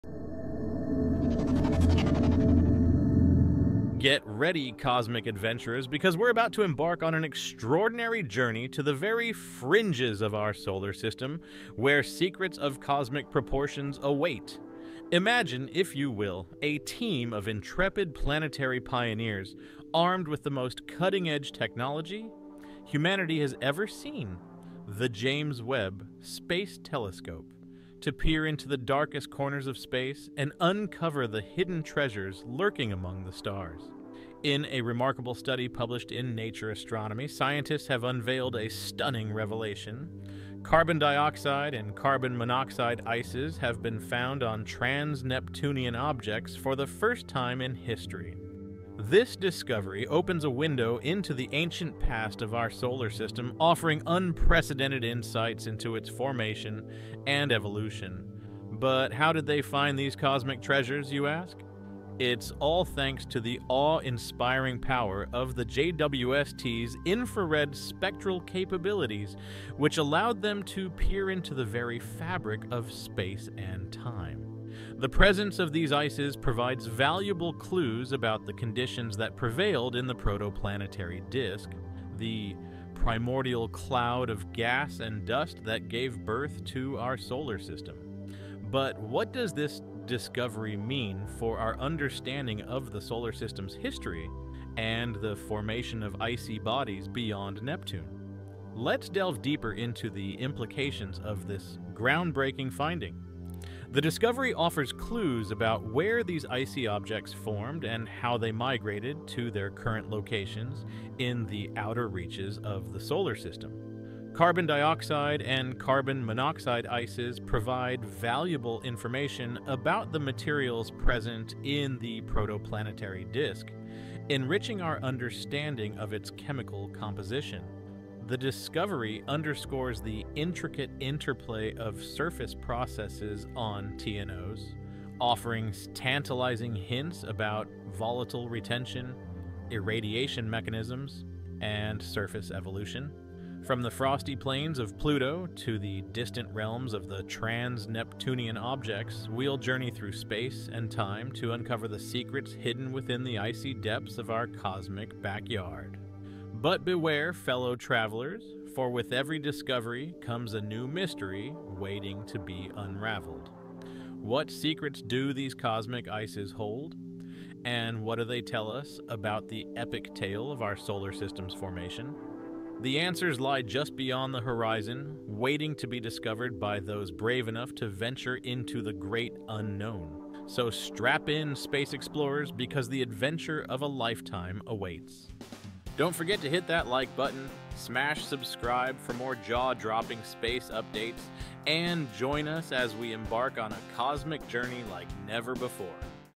James Webb Space Telescope Finds Hidden Treasures in the Outer Solar System!
In a remarkable study published in Nature Astronomy, scientists have unveiled a stunning revelation—carbon dioxide and carbon monoxide ices have been found on trans-Neptunian objects for the first time in history. This groundbreaking discovery was made possible by the incredible capabilities of the James Webb Space Telescope (JWST). By peering into the darkest corners of space, the JWST has opened a window into the ancient past of our solar system, offering unprecedented insights into its formation and evolution...